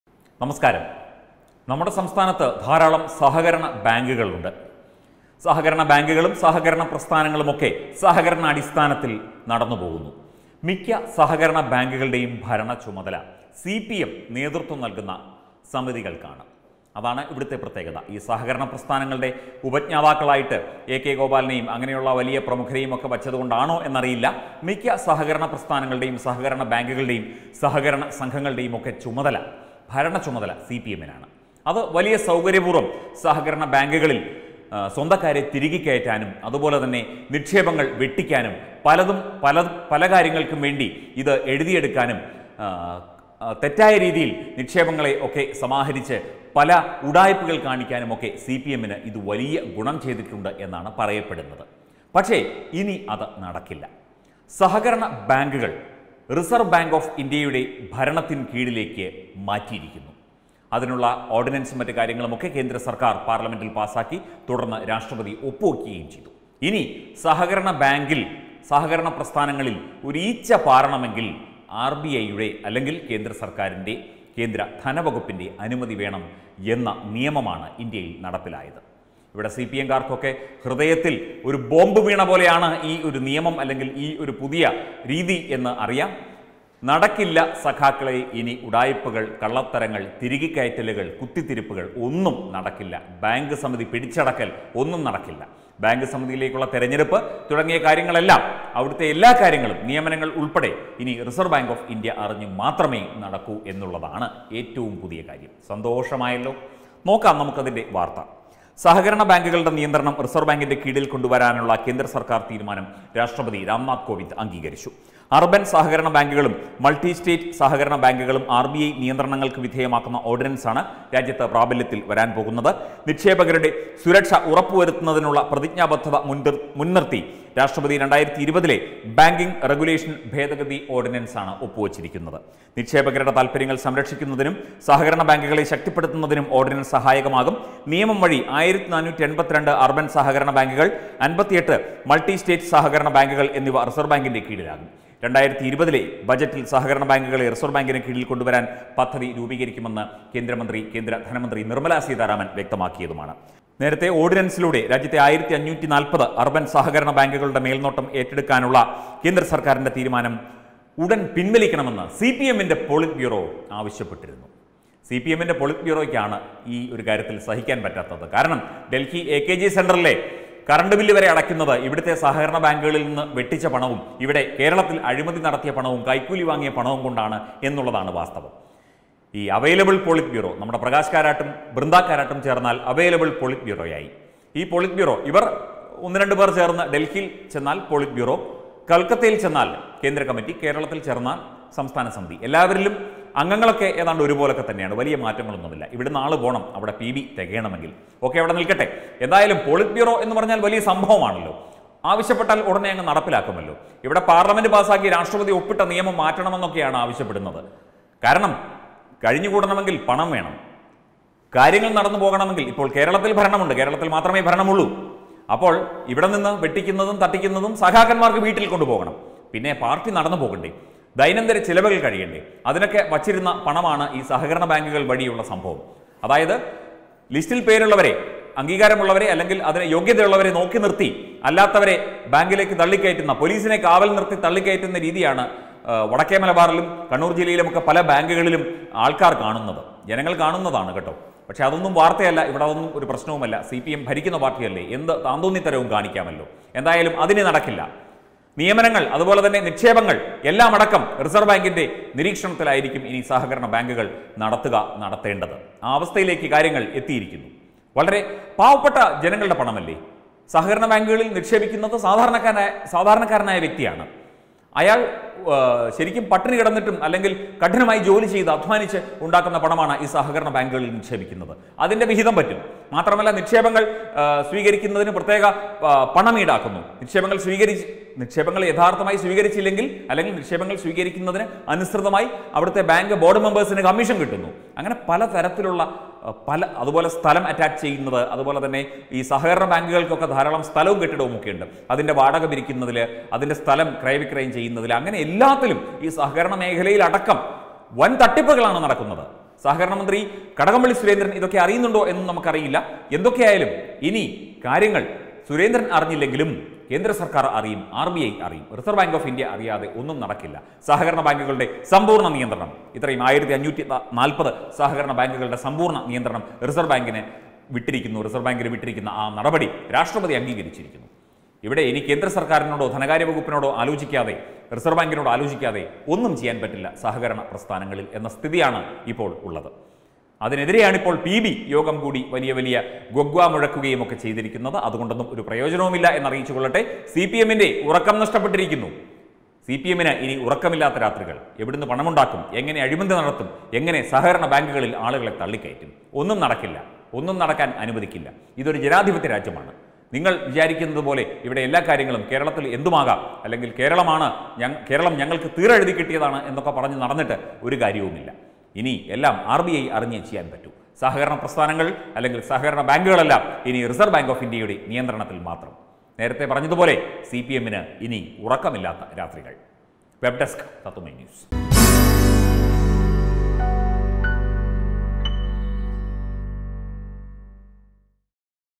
സിപിഎമ്മിന്റെ ഉറക്കം പോയി....ഇനി എങ്ങനെ തട്ടിക്കും വെട്ടിക്കും,തിരുകിക്കയറ്റും...എല്ലാം സ്വാഹ...
സഹകരണ ബാങ്കുകളുടെ ഭരണവും നിയന്ത്രണവും ഇനി ആർ ബി ഐക്ക്.നിയമം പാസായി.കേരളത്തിലെ സി പി എം ഗോവിന്ദ ഗോവിന്ദ...